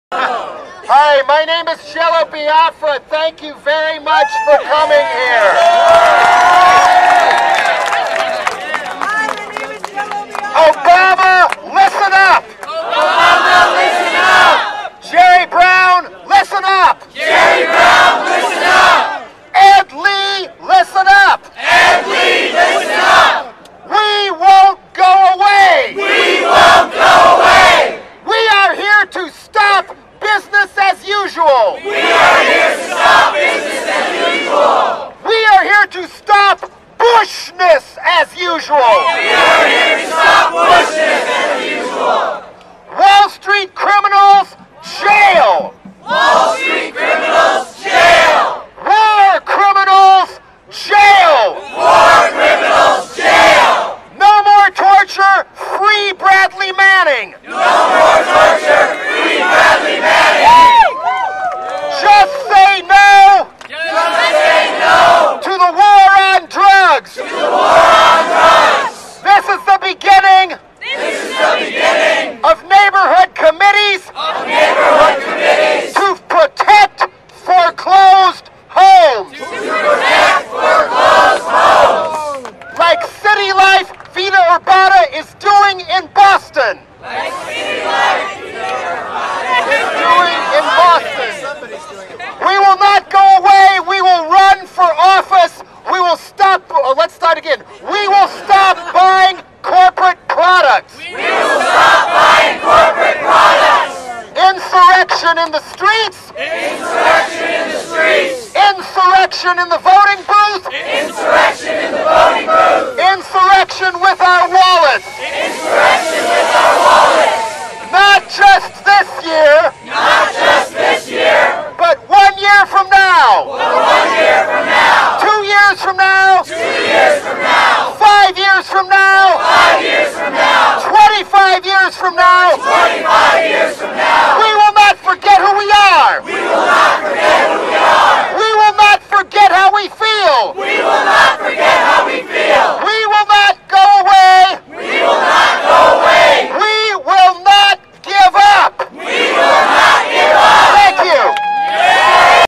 Occupy San Francisco: Insurrection in the Streets!
Occupy San Francisco Speaker is angry at Democrats and wants war criminals prosecuted.